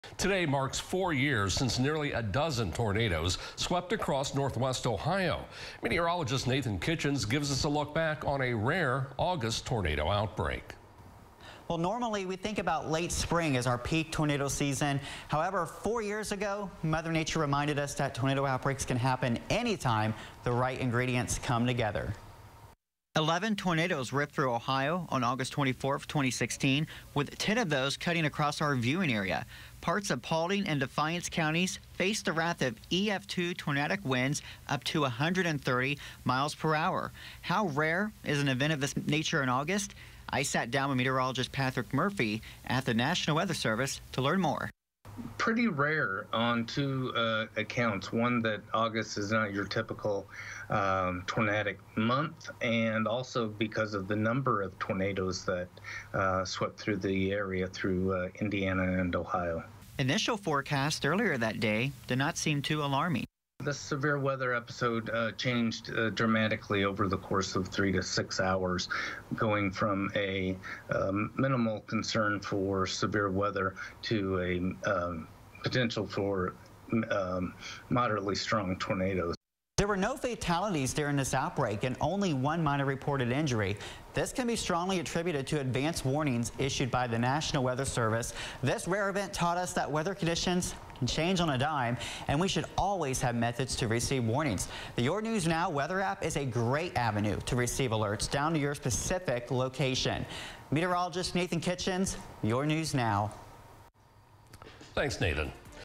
Anniversary of August 2016 Tornadoes
Some unusual weather 4 years ago today when 22 tornadoes swept across Indiana and Ohio, making for the largest single-day tornado outbreak in August. You likely remember the flattened Starbucks in Kokomo where an EF-3 tornado touched down. Another EF-3 touched down near Woodburn, IN, with several EF-2's in northwest Ohio.